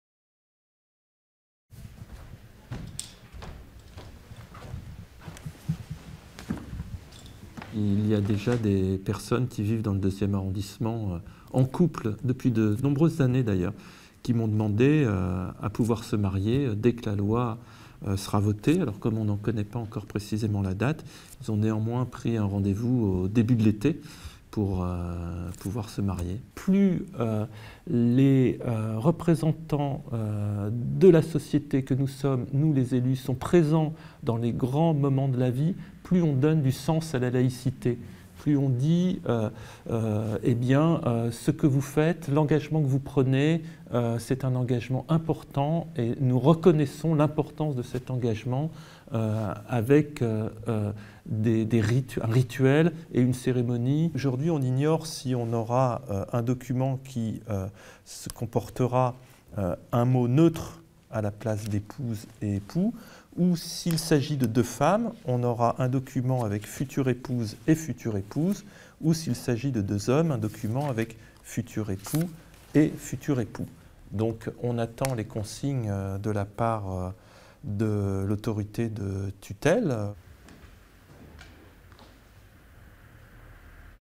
Mariage homosexuel: déjà des demandes en mairie
A la veille de l'examen à l'Assemblée Nationale du projet de loi sur le mariage pour tous, le maire du IIe arrondissement de Paris, Jacques Boutault, affirme que certains de ses administrés lui ont déjà "demandé à pouvoir se marier dès que la loi sera votée." Durée: 01:24